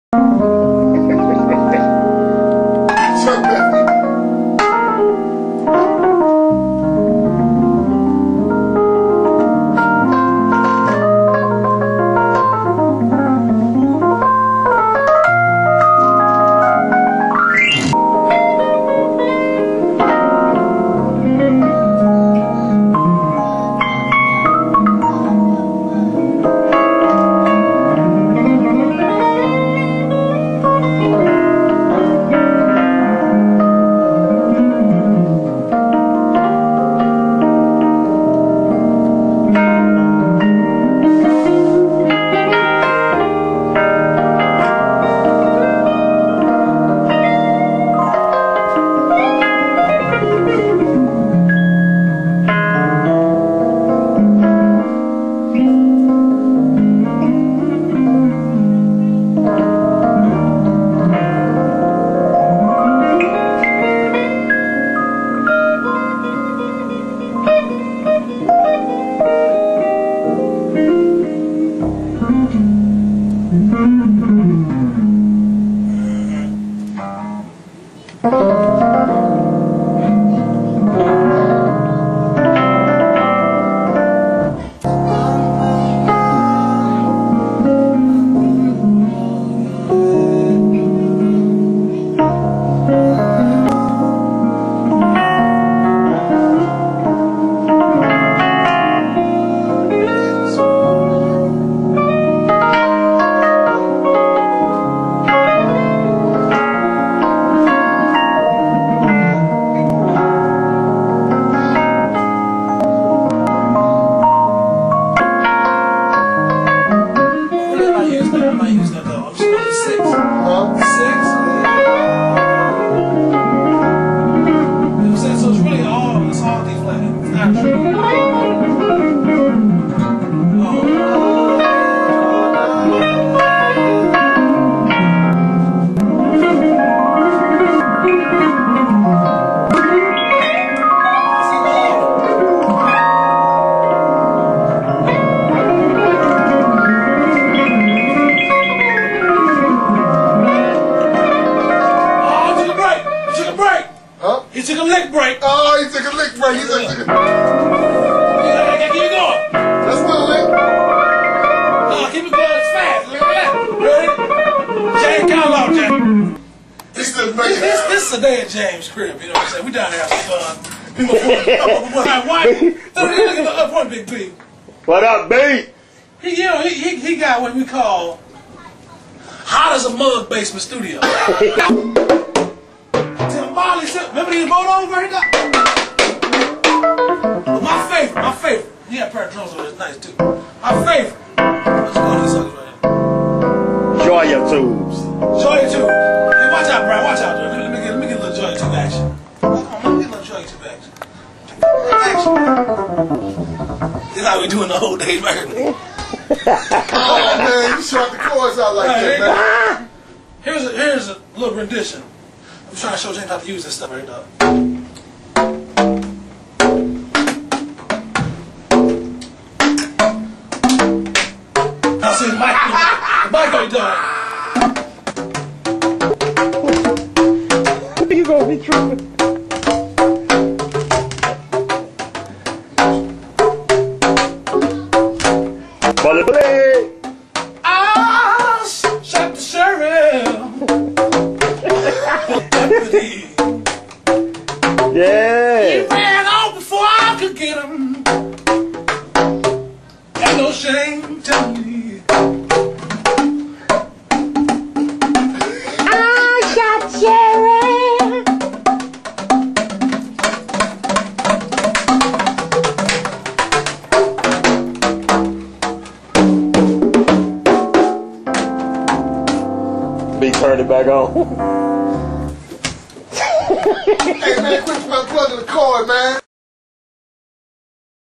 James Ross @ Chuck Bethany (Keyboards & Joia Tubes)
My boy Chuck Bethany(Fred Hammond)
& Brian White (Guitar)clowning around
in the bass-ment.